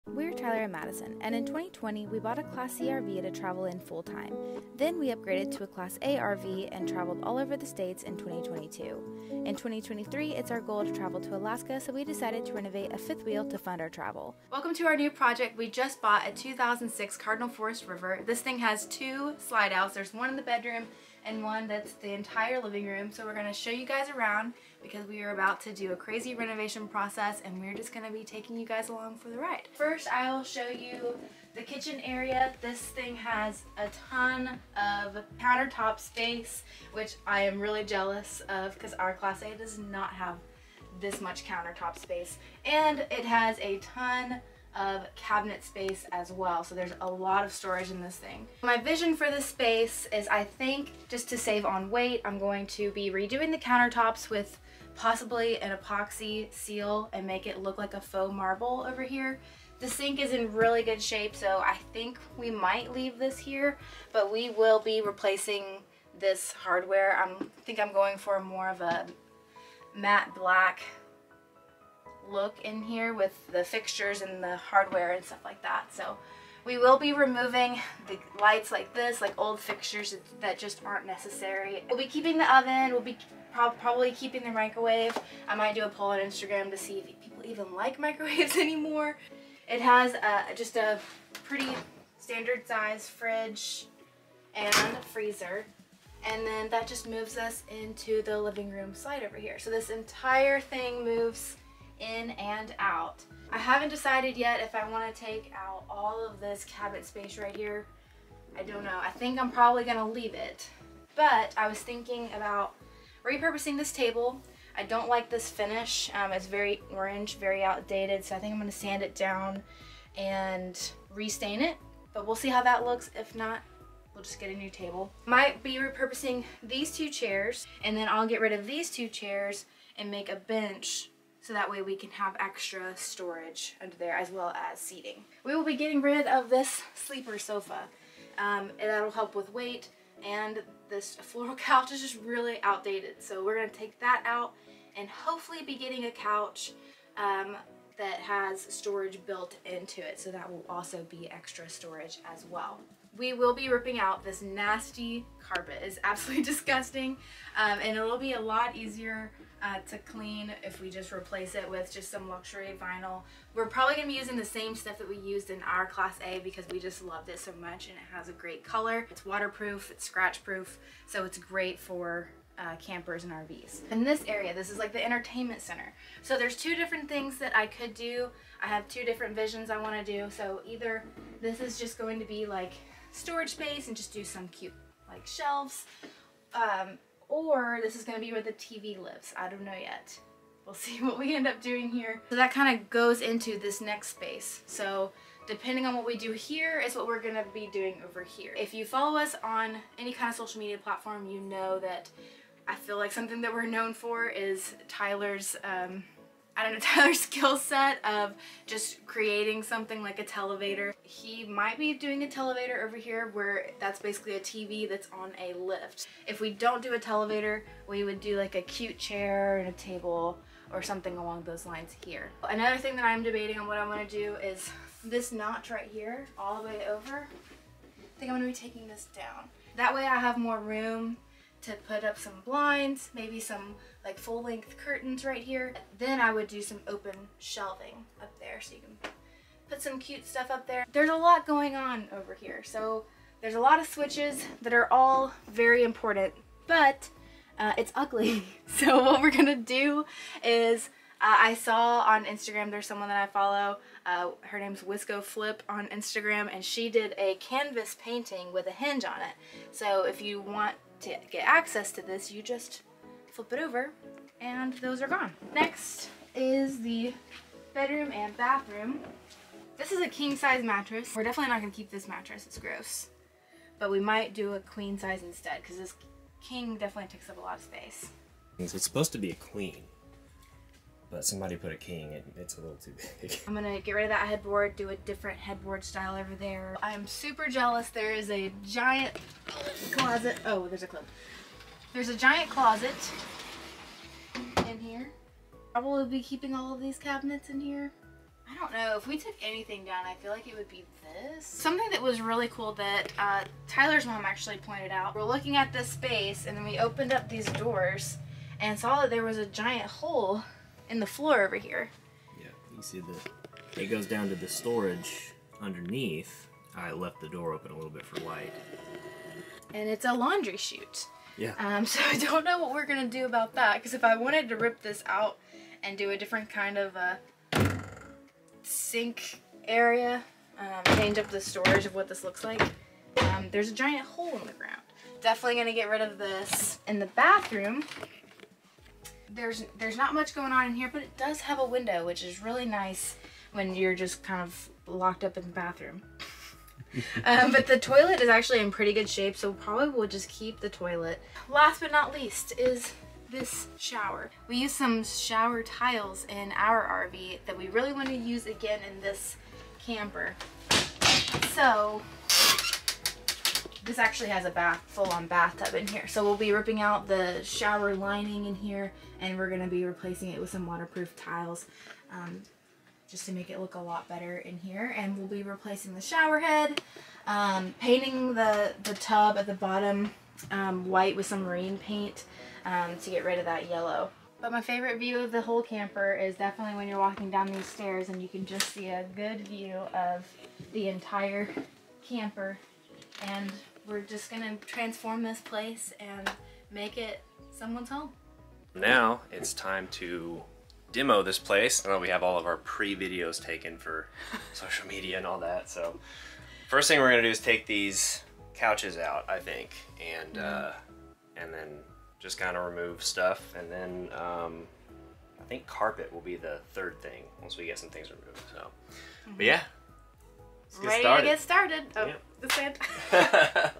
Pre Renovation Tour of a 2006 Cardinal Forest River Fifth Wheel!
Check out this Pre Renovation Tour of a 2006 Cardinal Forest River Fifth Wheel!

We plan on renovating this camper to turn it into someones home on wheels. Once we sell it in May, we will be hitting the road to head to Alaska!